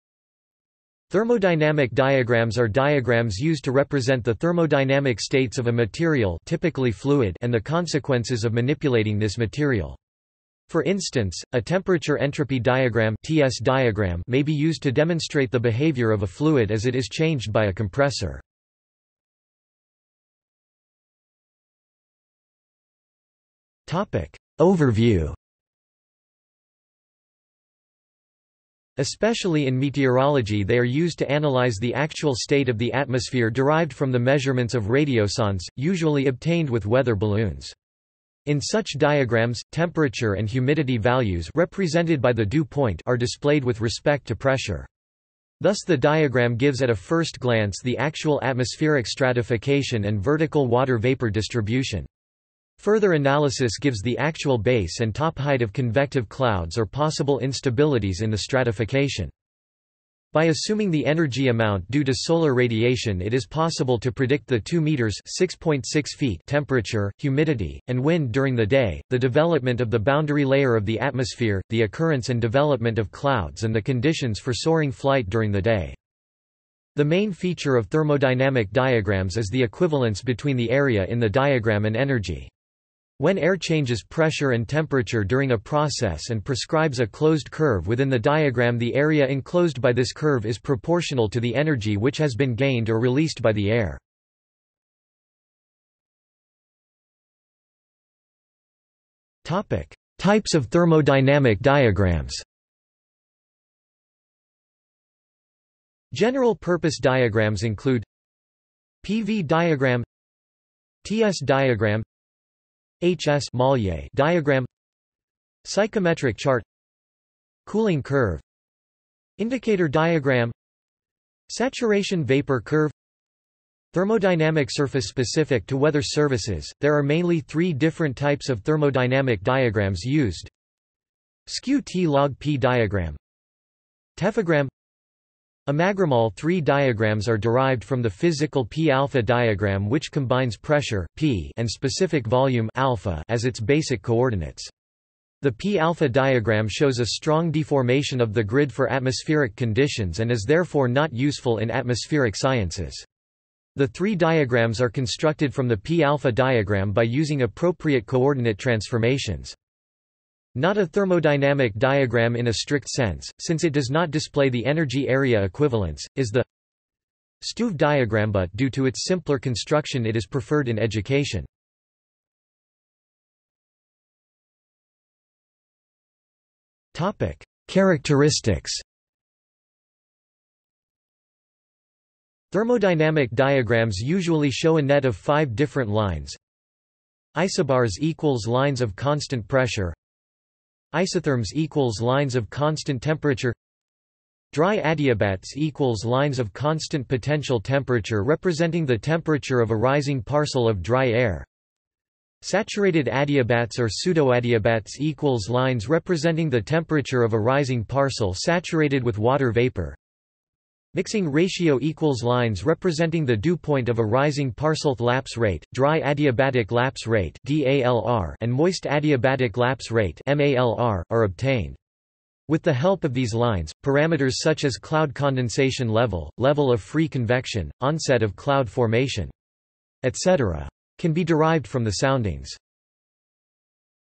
Thermodynamic diagrams | Wikipedia audio article
00:00:20 1 Overview
00:01:35 2 Types of thermodynamic diagrams
00:02:53 3 Characteristics

- Socrates

SUMMARY
Thermodynamic diagrams are diagrams used to represent the thermodynamic states of a material (typically fluid) and the consequences of manipulating this material.  For instance, a temperature–entropy diagram (T–s diagram) may be used to demonstrate the behavior of a fluid as it is changed by a compressor.